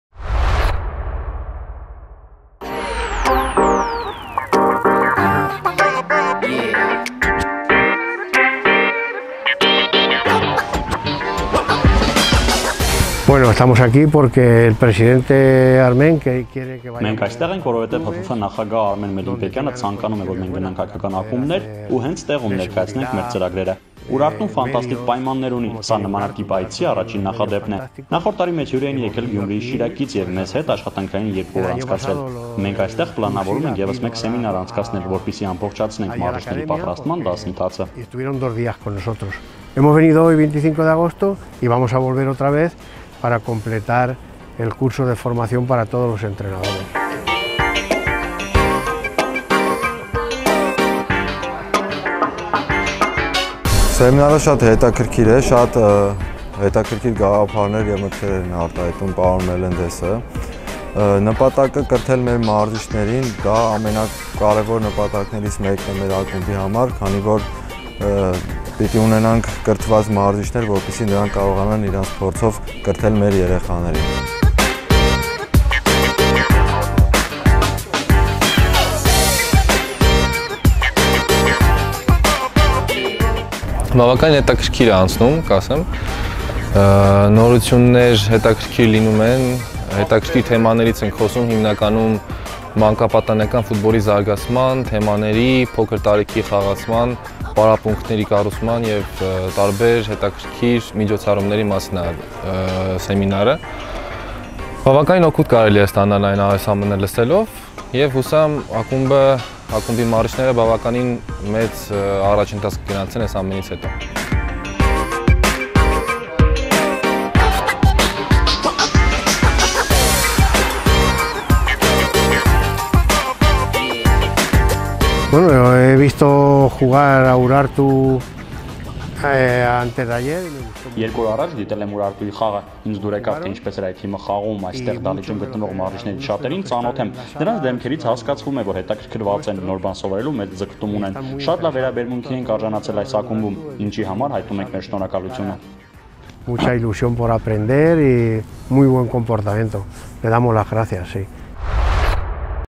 GINEZ MELENDEZ AND ANTONIO FLORES HELD SEMINAR WITH COACHING STAFF OF URARTU FC
Technical director of FFA Ginez Melendez and head of Educational program Antonio Flores held a seminar with coaching staff of Urartu FC.